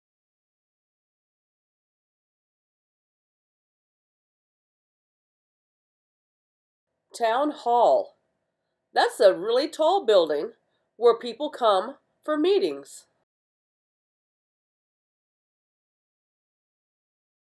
Mike Mulligan and His Steam Shovel - Townhall
This video is part of the Lesson Plan on "Mike Mulligan and His Steam Shovel" by Virginia Lee Burton. English captions and English voiceover are available.